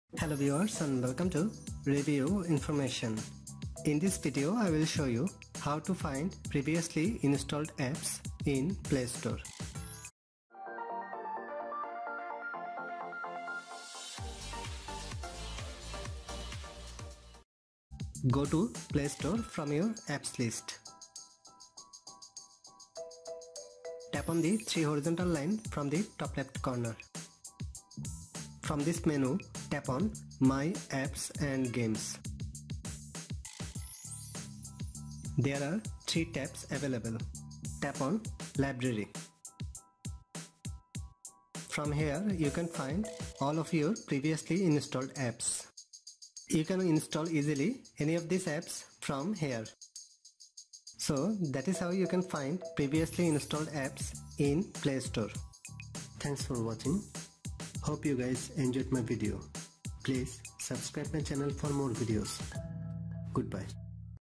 How to Find Previously Installed Apps on Google Play Store
In this video I will show you, How to Find Previously Installed Apps on Google Play Store.